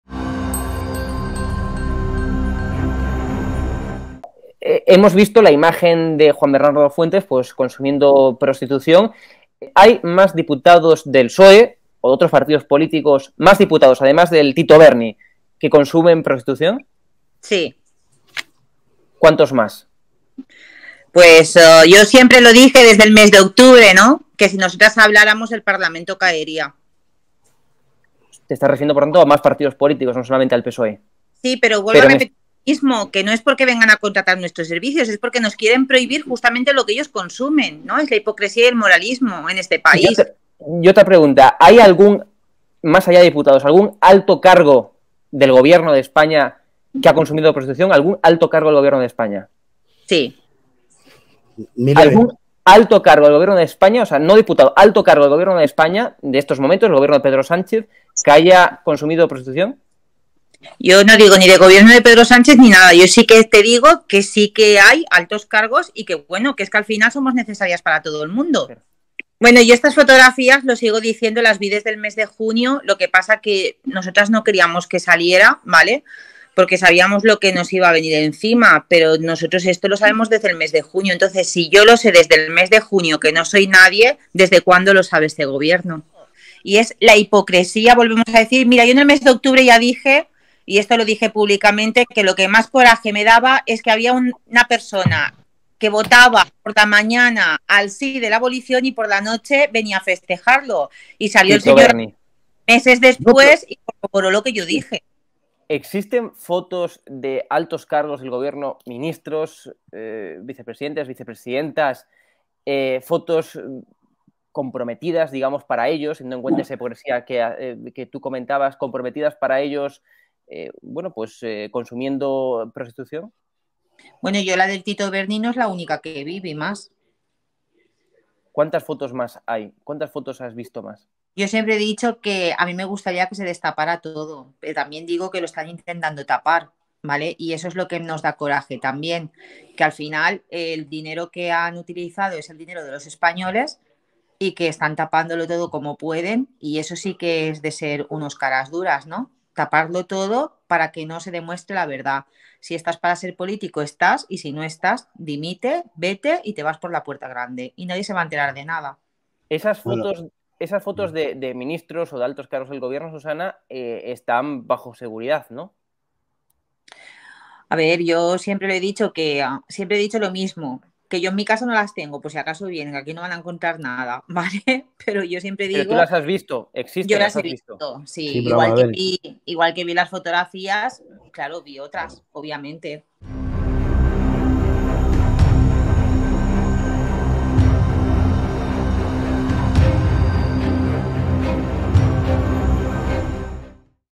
El consumo de prostitución en PSOE y Podemos: siniestro robo de 20 pendrives
Susana Pastor denunció en la Policía Nacional el robo de, tan solo, 20 pendrives. Todo tras asegurar hace pocas semanas en ESdiario que existen imágenes de políticos de PSOE y Podemos consumiendo prostitución.